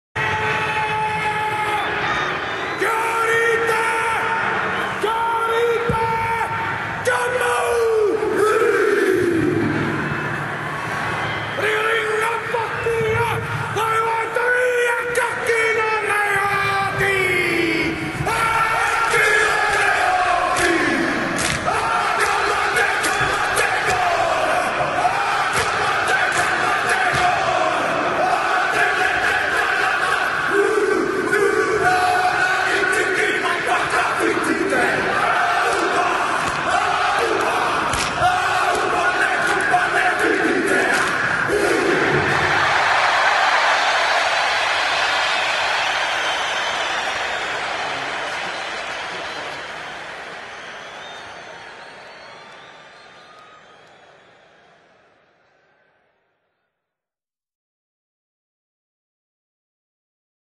IMAGES CHOCS: Le Haka-gerbe de rapta (interdit -18)
"Trés" court-métrage:
Le Haka-gerbe de rapta, un vrai guerrier comme on en voit plus.